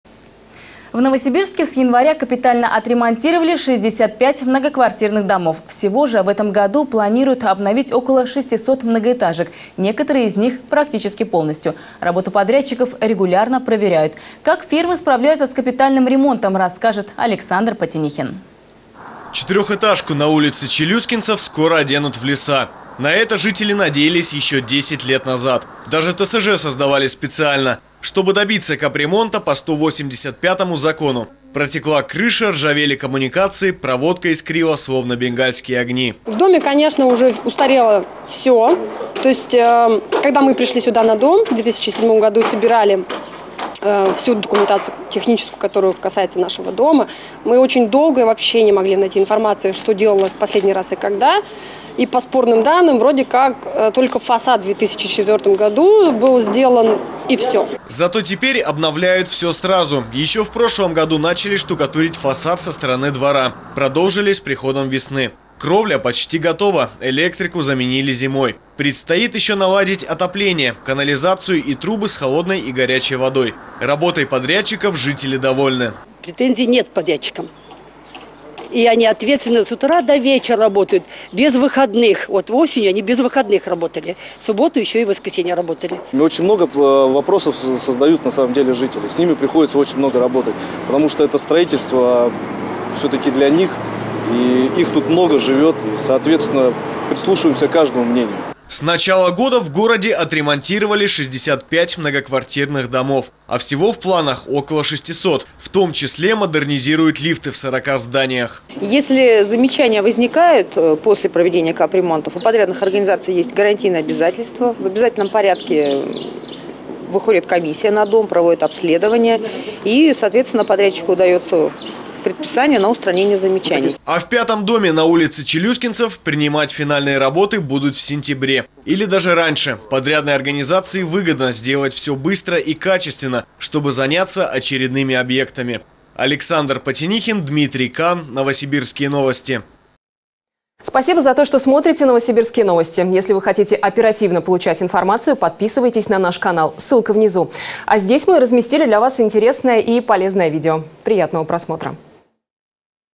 Как проводится капитальный ремонт, проверили в Новосибирске
В Новосибирске с января капитально отремонтировали 65 многоквартирных домов, всего же в 2017 году запланирован капремонт порядка 600. Инспекции регулярно проверяют работу подрядных организаций.Все новости Новосибирска на сайте "Новосибирские новости"